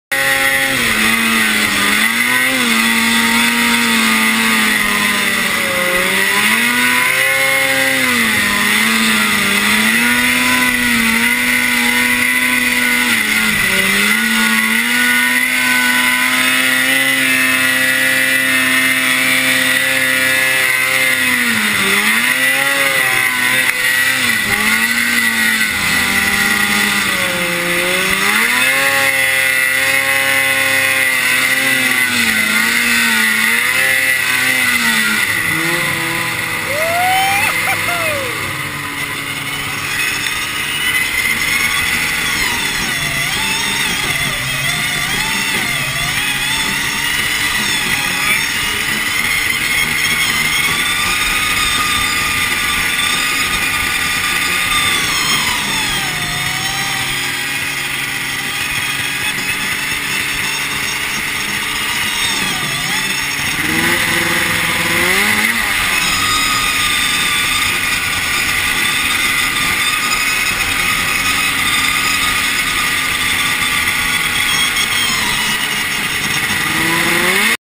Mountain Climbing with my snowmobile. Kodiak AK
1999 Polaris Indy RMK 700.  Looks like its in rough shape, but it performs awesome.